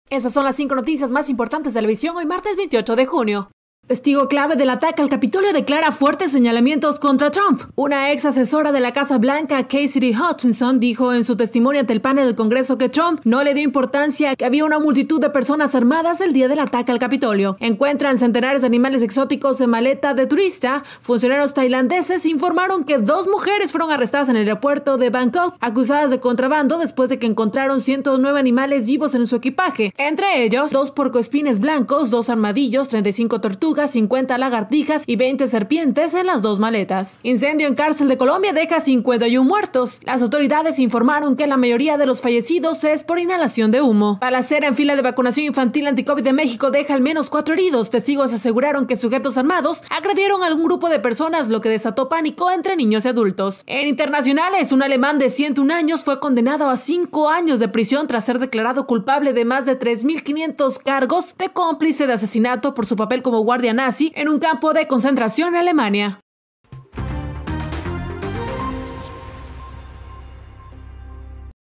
¡Insólito! Encuentran animales en maleta de turista
🟣 00:04 Testigo clave del ataque al capitolio, declara fuertes señalamientos contra Trump

🟣 00:18 Encuentran centenares de animales exóticos en maleta de turista

🟣 00:37 Incendio en cárcel de Colombia deja 51 muertos

🟣 00:44 Balacera en fila de vacunación infantil antiCOVID en México  deja al menos 4 heridos

🟣 00:54 Condenan a un guardia nazi de 101 años

Disfruta nuestra gran variedad de  programas en nuestra señal  de radio en streaming